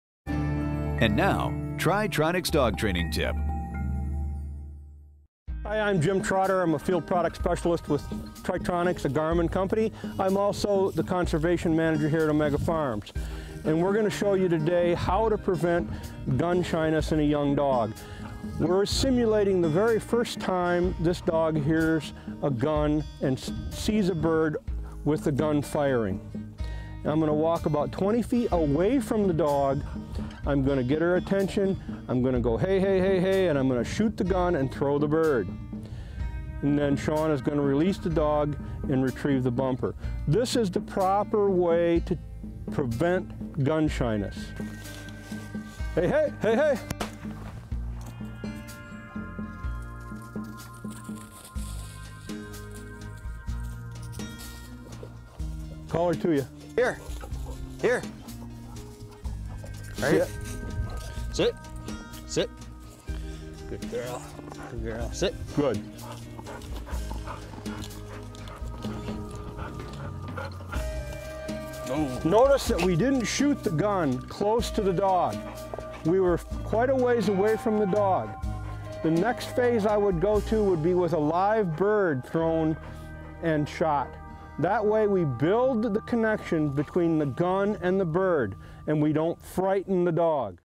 American BirdHunter - TriTronics :: Gun Shyness
Jim Trotter from American Birdhunter and Tri-Tronics talks about the proper way to train your retriever for hunting.